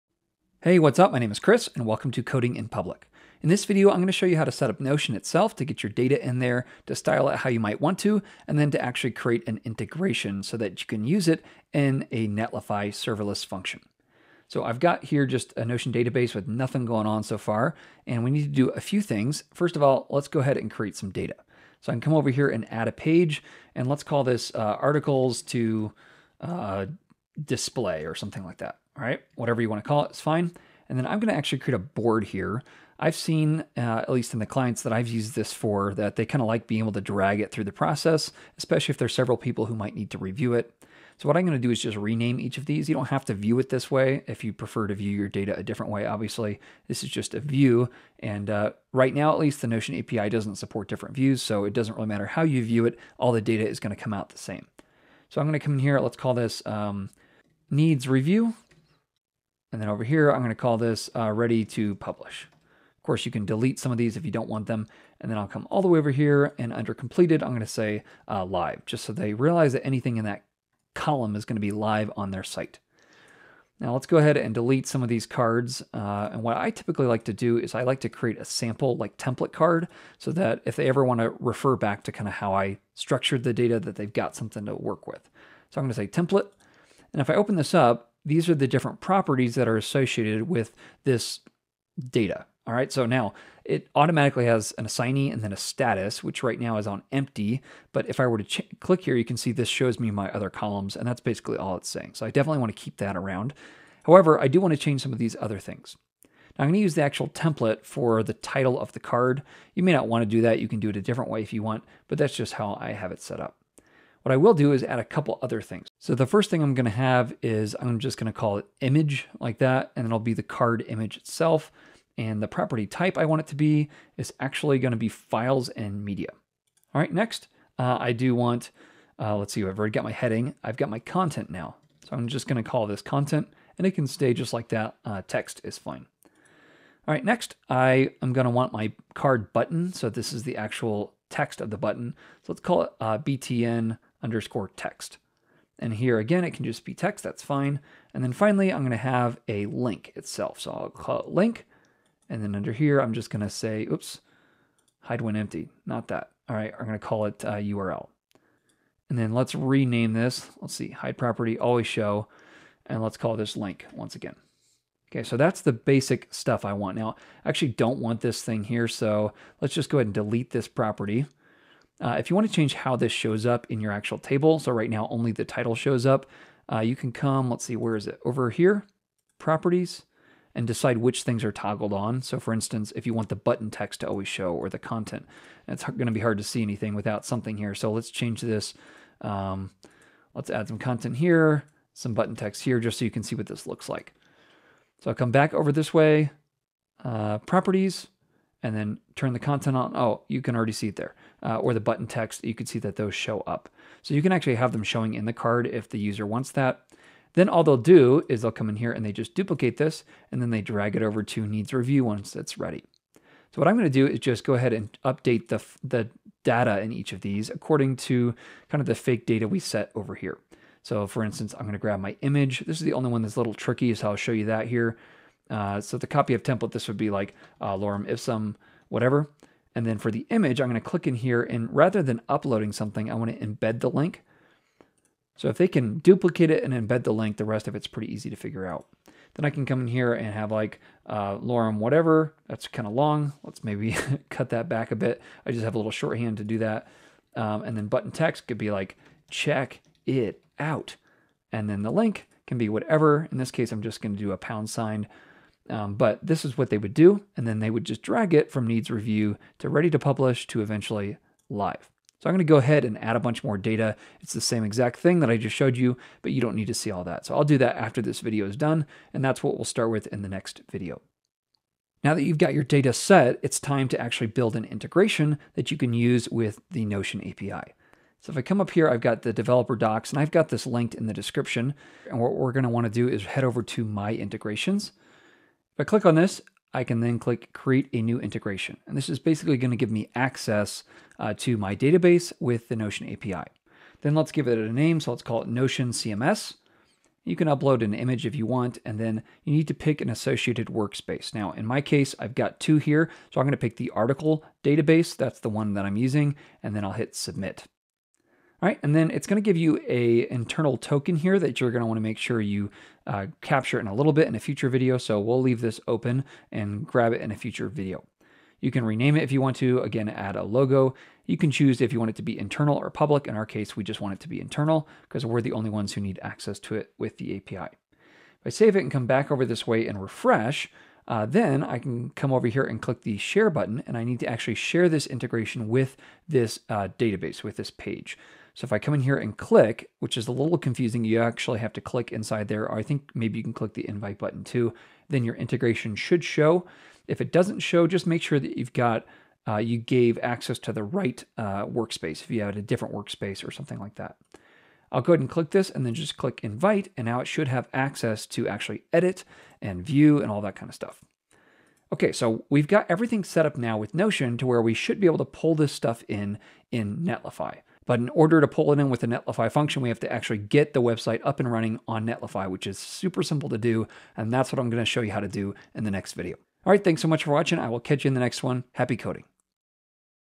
How to Use Notion as a CMS (Part 3: Setting up Notion Data and API Integration)
Notion is a powerful online database tool with nearly infinite flexibility. For this reason, it’s attracted dedicated users since it was first launch. In 2021, it published a public Notion API in beta, making it yet more accessible and customizable! In this video series, I'll show you how to use Notion as a light-weight CMS. While not ideal for scaffolding out an entire site, it can act as a very capable CMS for users with mostly static content. For the one or two sections on your site that need regular updating, why not use something like Notion?

I'll show you how to:
1. Create the basic site with ViteJS and Open Props using HTML and CSS
2. Set up Notion and create and add an API Integration
3. Publish your site to Netlify for free
4. Create a Netlify serverless function with the Notion SDK for JavaScript and the Netlify CLI (including environmental variables)
5. How to fetch the live Notion data from your Netlify serverless function
6. Write a skeleton loading animation to improve the site UX

⏲️  Timestamps ⏲️
0:00 Introduction
0:18 Add data to Notion
4:31 Adding a Notion integration